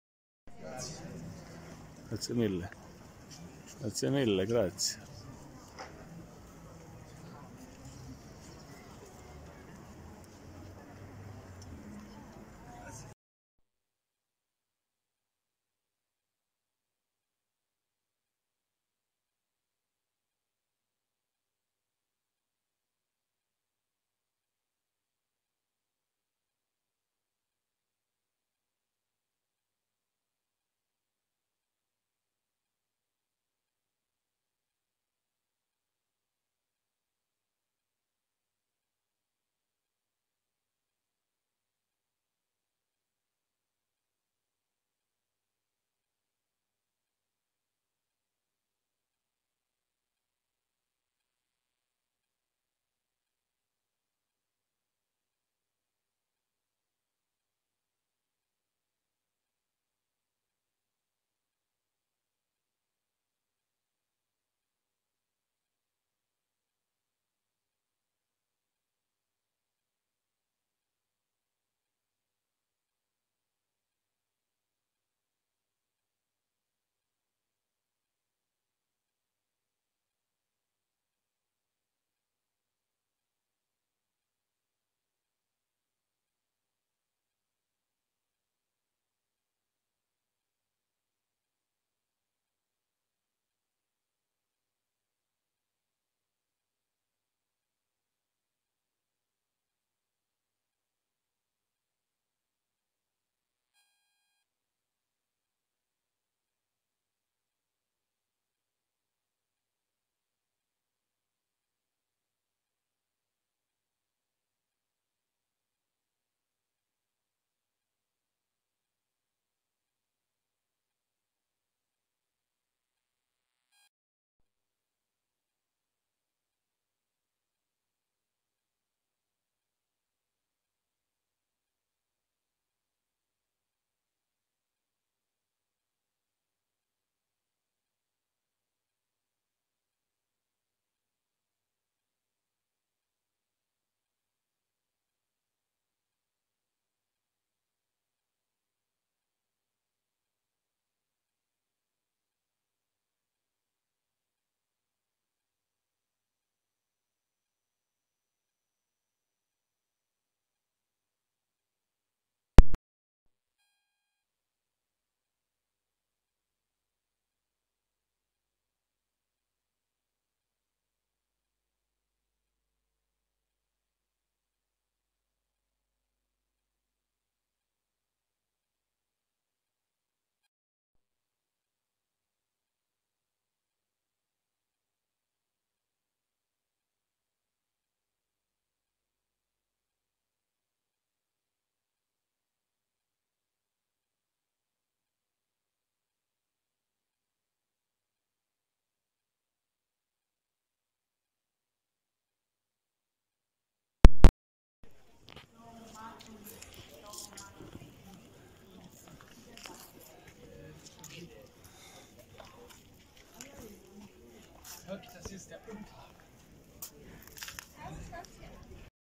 Walking in beautiful Taormina center.
Walking in beautiful Taormina center. Region of Sicily. Magical view of the Sicily coast